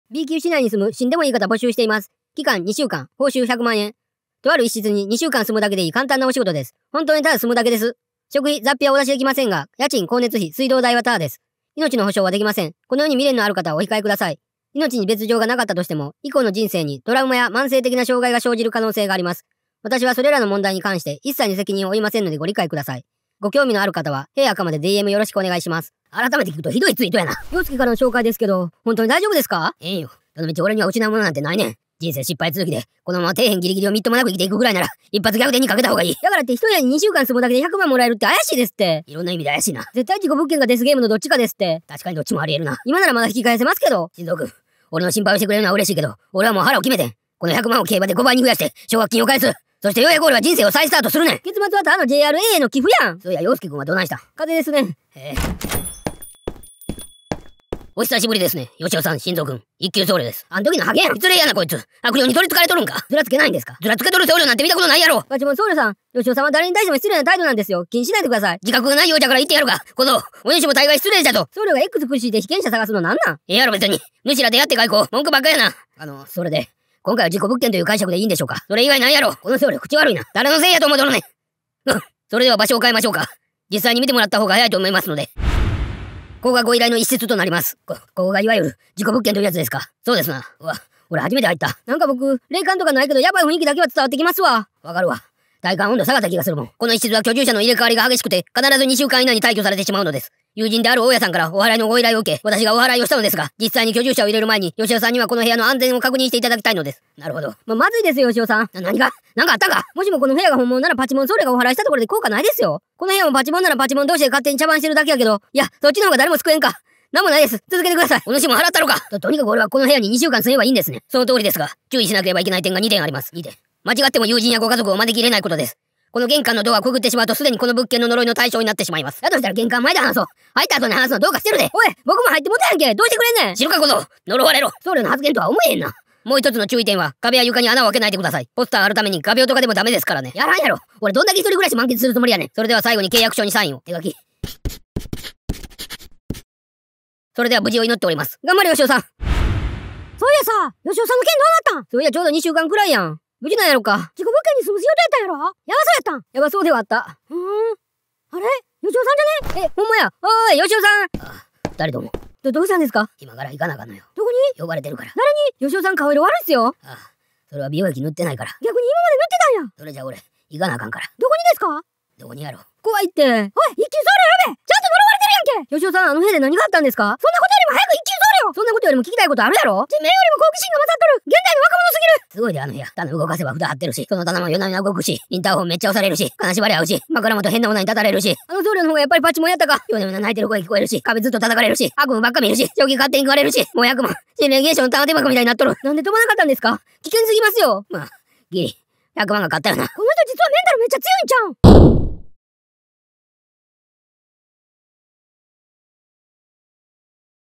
事故物件に2週間住むだけで100万円もらえる仕事に挑む男【アニメ】
みんなが知っているあるあるではなく、陰のあるあるをテーマにした
見たら元気になれる自作ショートアニメです。

LINEスタンプ出ました！
ご購入はこちらから